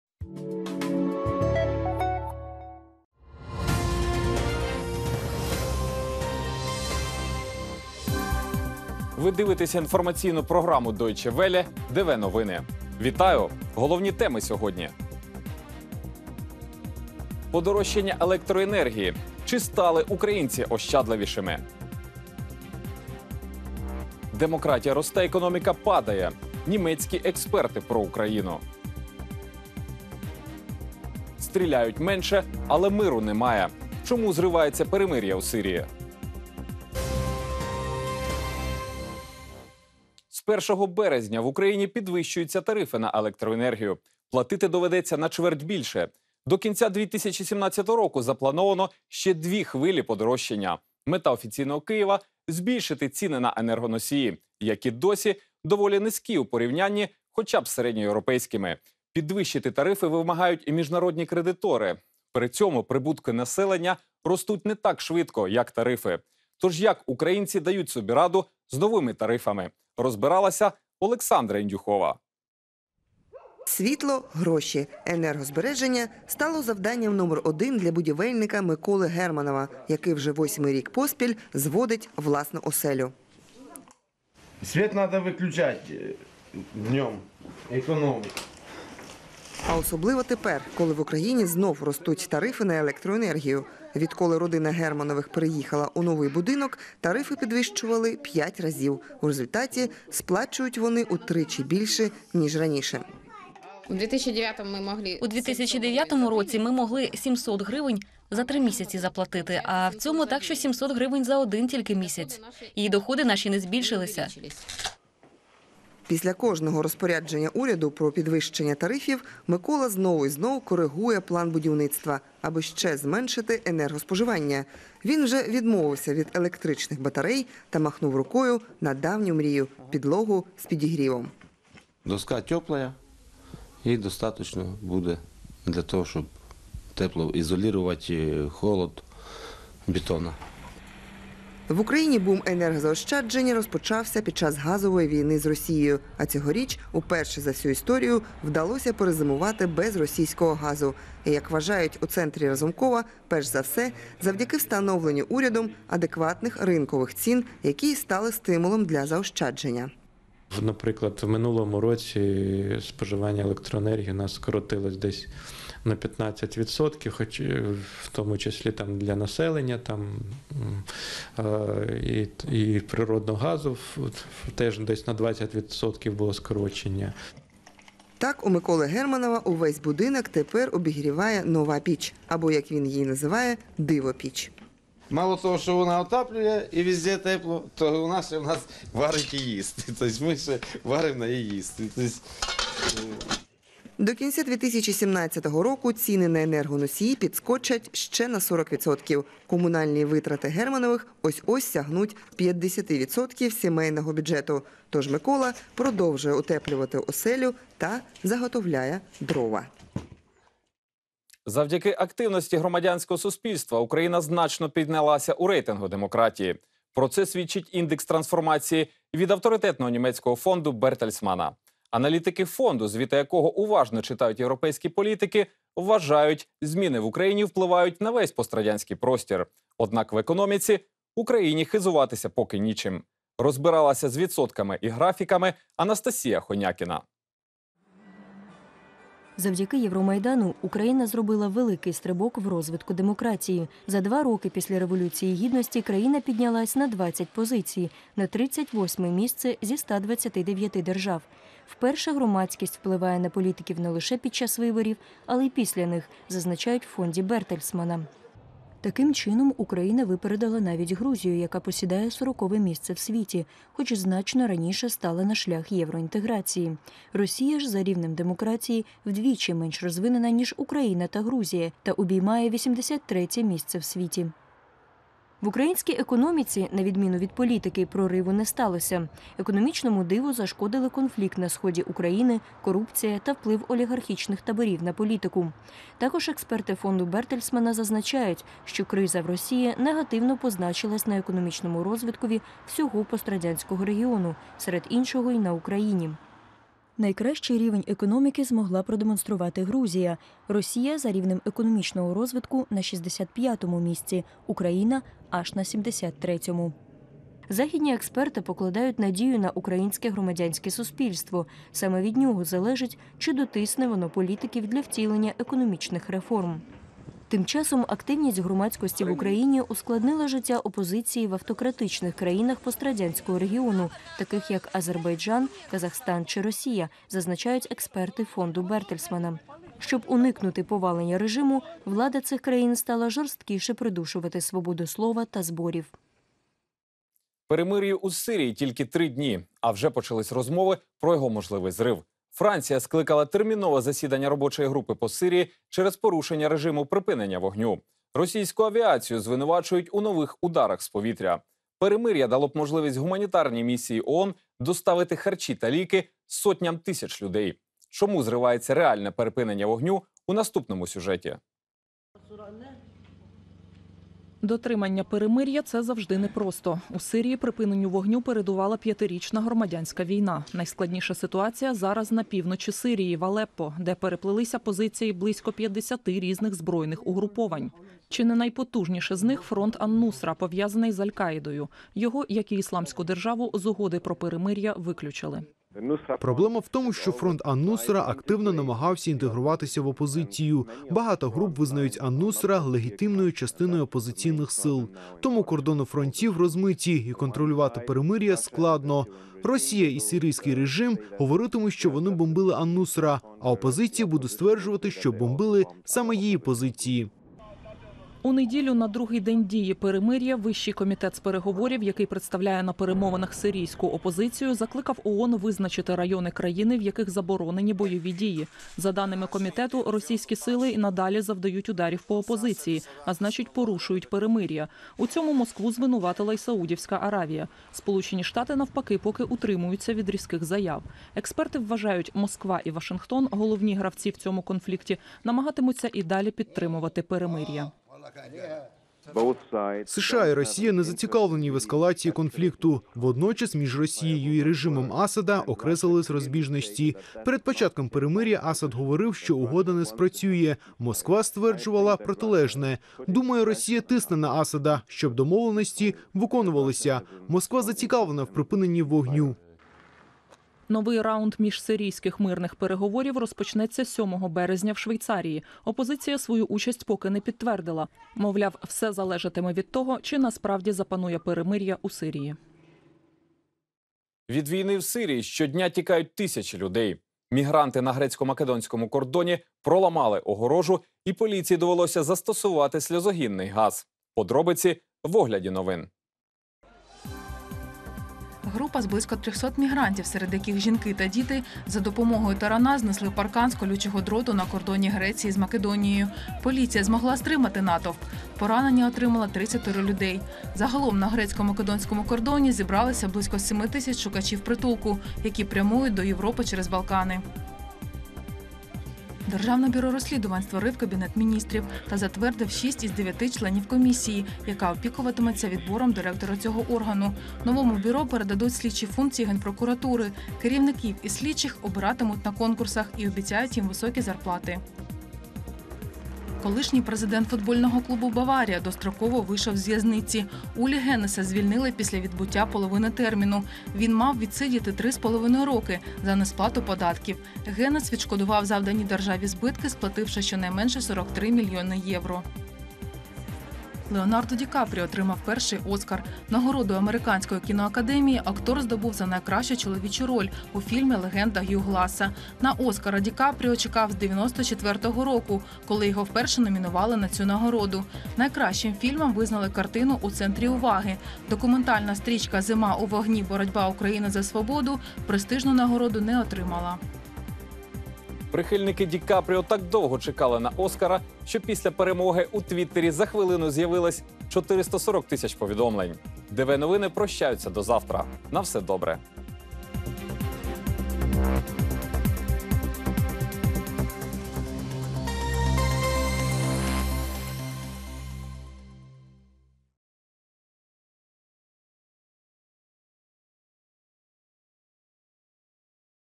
Німецьке дослідження: українська демократія росте, економіка падає - DW Новини (29.02.2016)
Індекс трансформації фонду Бертельсманна.
Українці стають усе більш ощадливими на тлі нового подорожчання електроенергії.
Чому зривається перемир'я у Сирії?